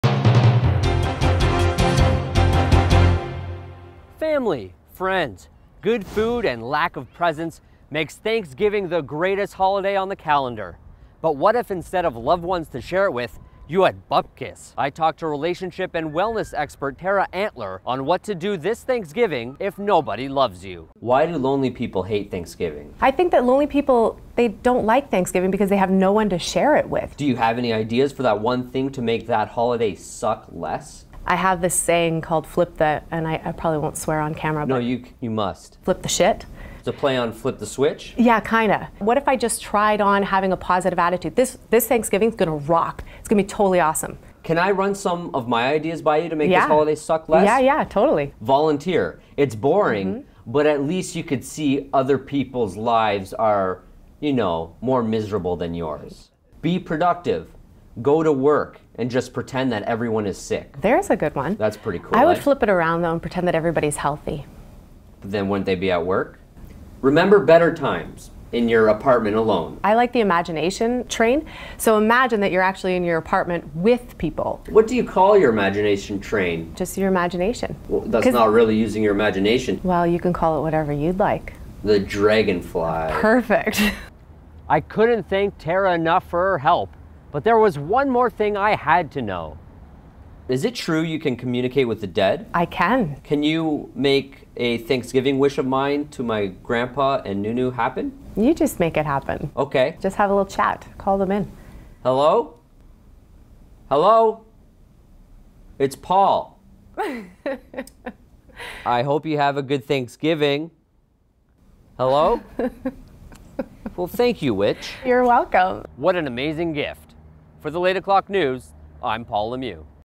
The Late O'Clock News - Tara Antler, Relationship Expert
I sat down with relationship expert Tara Antler (taraantler.com)  to talk loneliness during Thanksgiving.